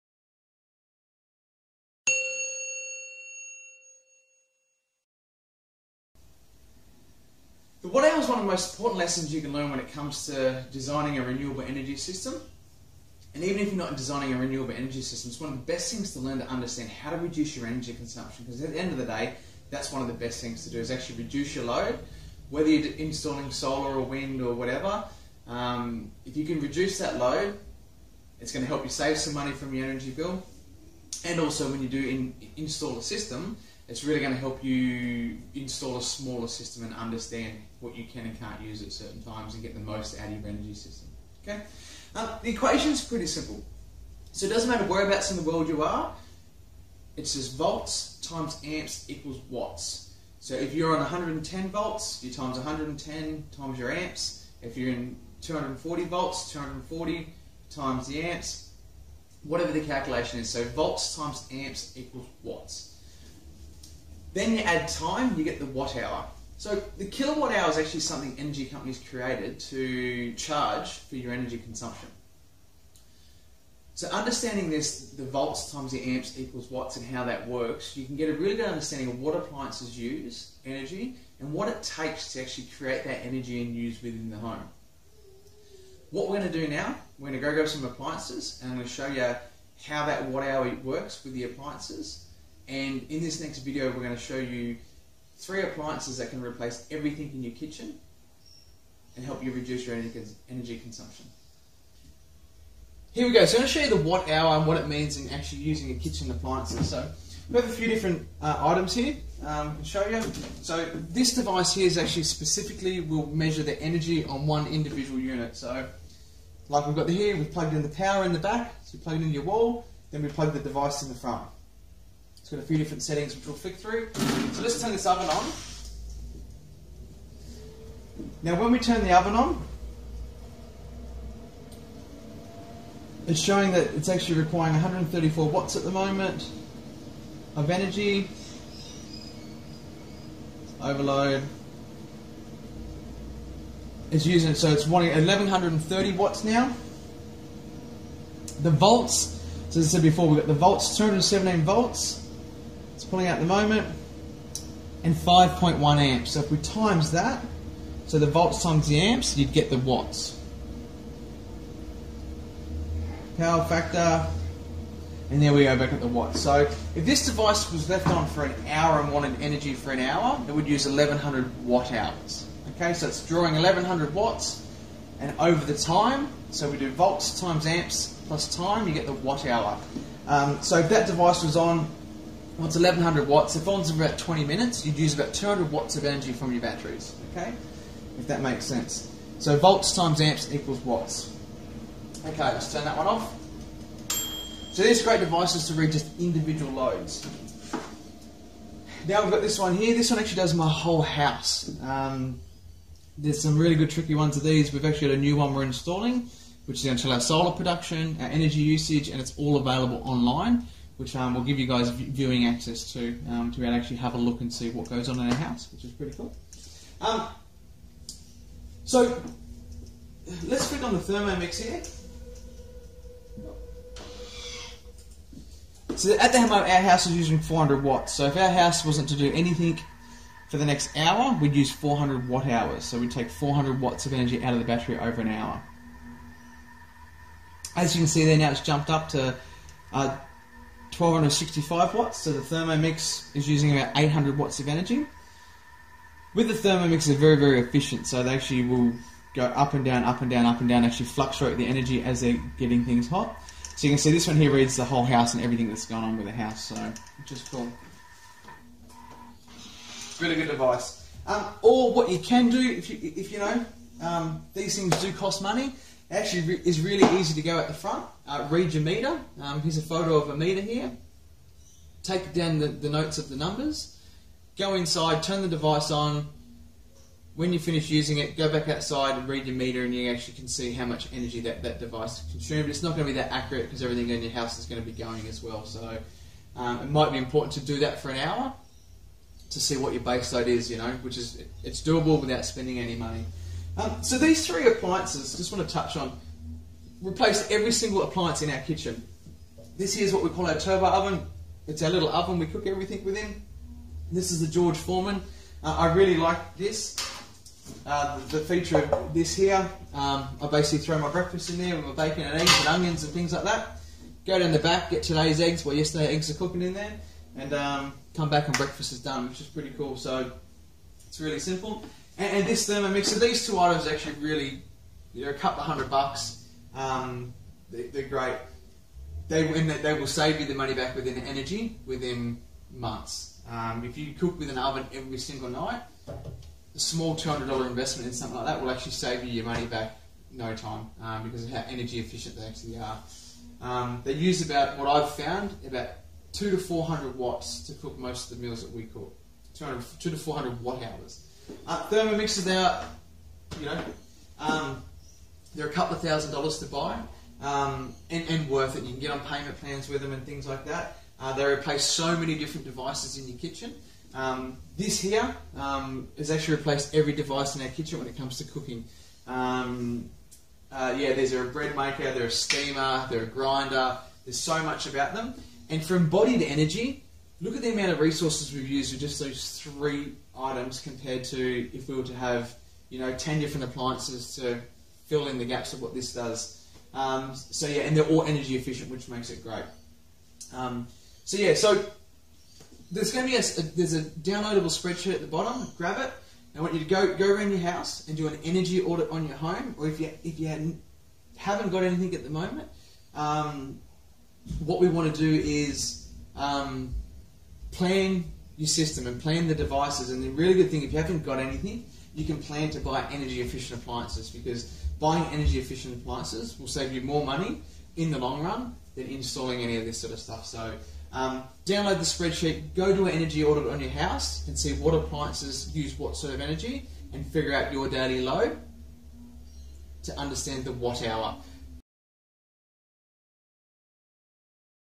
THE WATT HOUR
The Watt-hour is the most important piece of information you can have when understanding energy. It will assist you in all your decisions in solar batteries and even buying your appliances.